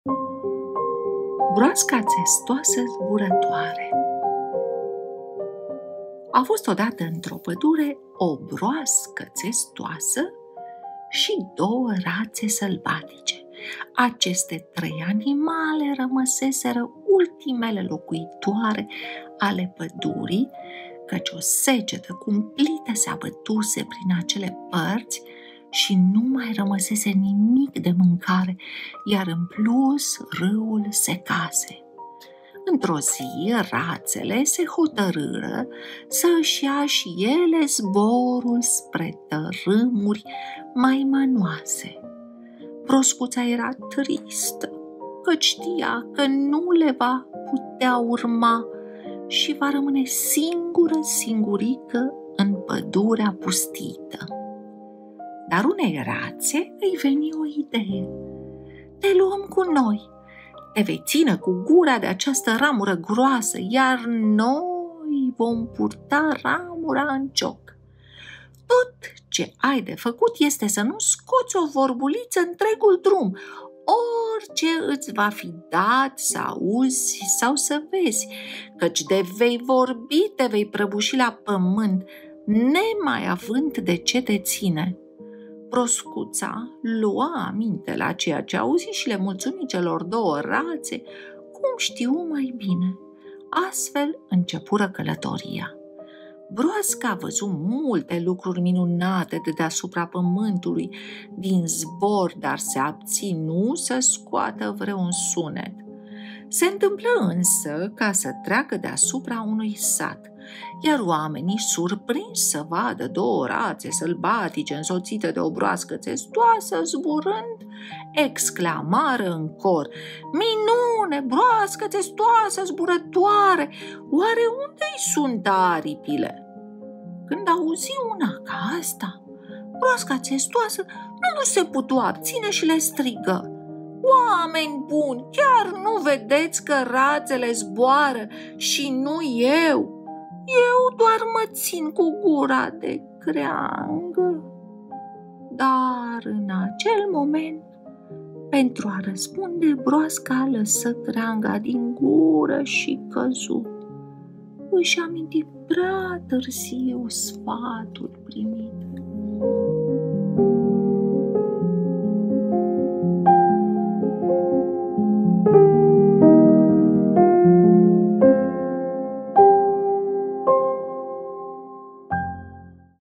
Broasca țestoasă zburătoare 🐢 Povești pentru copii în limba română | Povești scurte 4K (151/365)
Povești noi cu broasca țestoasă zburătoare pentru copii. Povești pentru copii în limba română. Povești scurte audio în 4K. Să citim o carte de poveste împreună. Basme și povești de lectura.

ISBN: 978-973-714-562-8